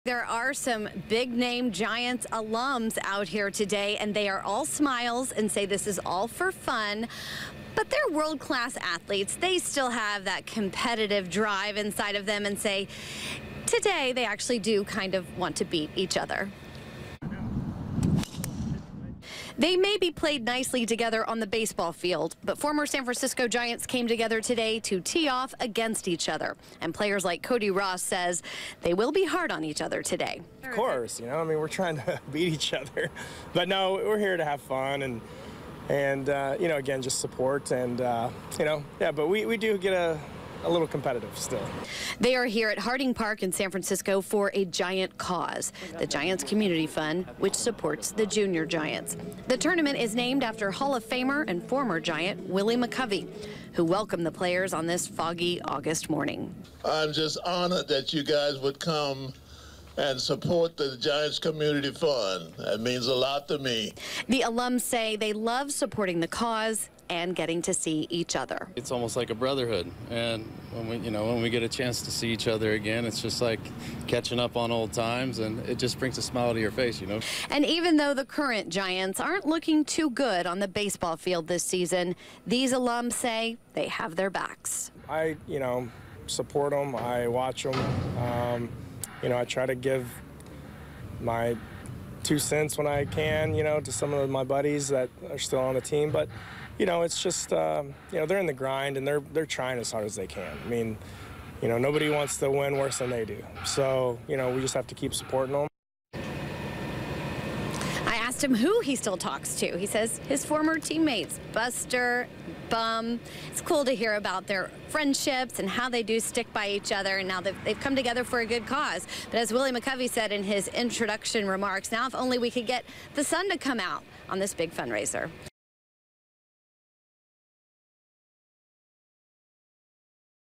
Giants alums compete in golf tournament to support children across West Coast
The Willie McCovey Golf Classic supports thousands of children across the West Coast and Giants players competed for a good cause.

-- Follow us for breaking news as it happens.